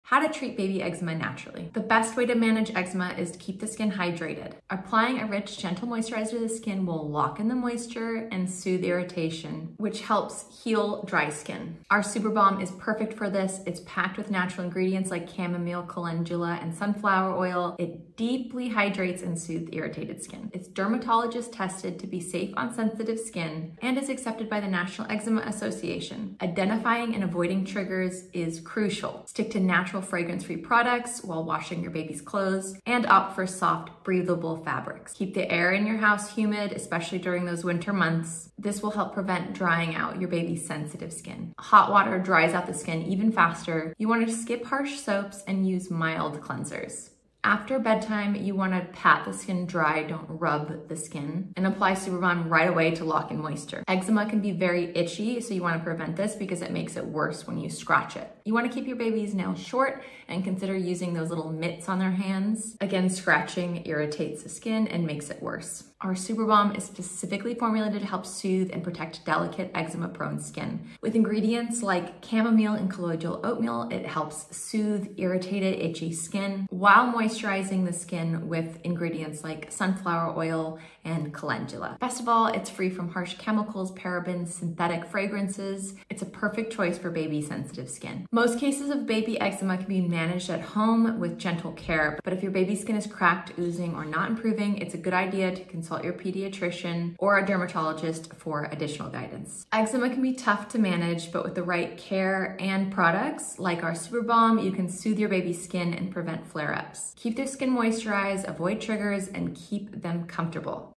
How To Treat Baby Eczema Naturally For New Moms
Dealing with baby eczema and irritated, dry skin? You're not alone, and there’s a natural way to give your little one the relief they need! In this video, we’re diving into how to treat eczema naturally with tips for soothing sensitive skin. Our go-to product? Era Organics Natural Baby Balm – an organic balm formulated to protect, moisturize, and calm baby skin affected by eczema, diaper rash, and more.

Why our Natural Baby Balm? Made with organic ingredients like chamomile, cocoa butter, and coconut oil, this balm provides deep hydration without harsh chemicals, making it perfect for delicate skin. Whether you’re managing eczema, cradle cap, or diaper rash, this balm is designed to soothe and restore. 🌿